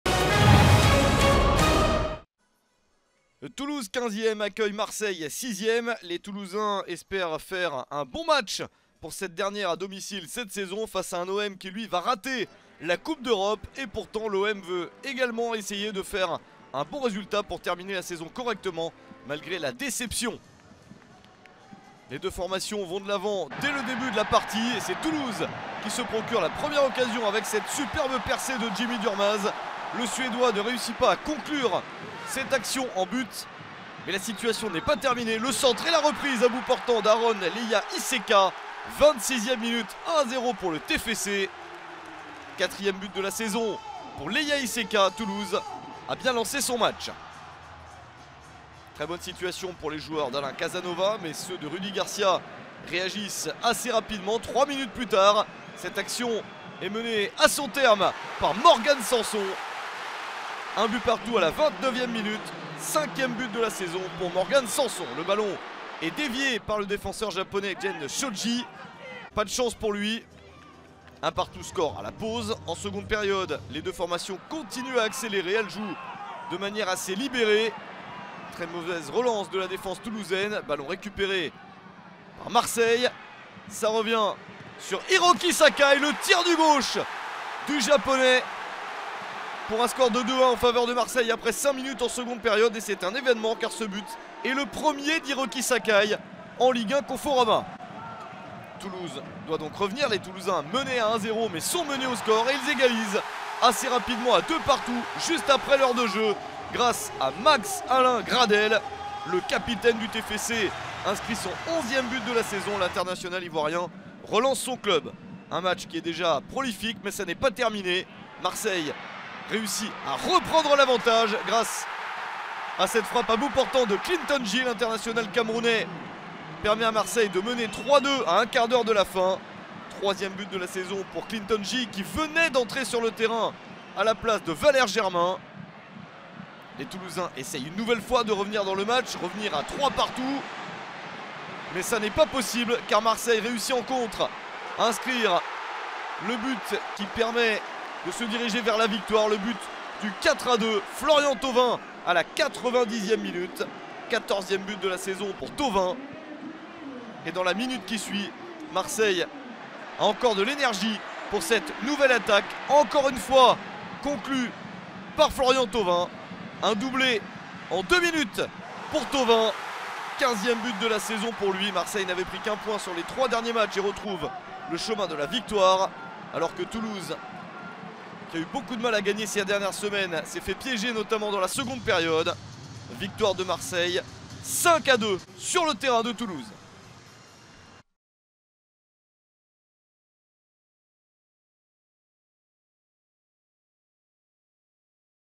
Toulouse FC - Olympique de Marseille ( 2-5 ) - Résumé - (TFC - OM) / 2018-19
Revivez les meilleurs moments de Toulouse FC - Olympique de Marseille (2-5) en vidéo.

Ligue 1 Conforama - Saison 2018/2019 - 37ème journée Stadium - samedi 18 mai 2019

Buteurs : Aaron LEYA ISEKA (26' - Toulouse FC) / Morgan SANSON (29' - Olympique de Marseille) / Hiroki SAKAI (50' - Olympique de Marseille) / Max-Alain GRADEL (61' - Toulouse FC) / Clinton NJIE (76' - Olympique de Marseille) / Florian THAUVIN (90' - Olympique de Marseille) / Florian THAUVIN (90' +1 - Olympique de Marseille)

Composition Toulouse FC : 1 - Mauro GOICOECHEA / 2 - Kelvin AMIAN / 3 - Gen SHOJI / 6 - Christopher JULLIEN / 7 - Max-Alain GRADEL (c) / 10 - Aaron LEYA ISEKA / 12 - Issiaga SYLLA / 14 - Matthieu DOSSEVI / 15 - John BOSTOCK / 17 - Ibrahim SANGARE / 21 - Jimmy DURMAZ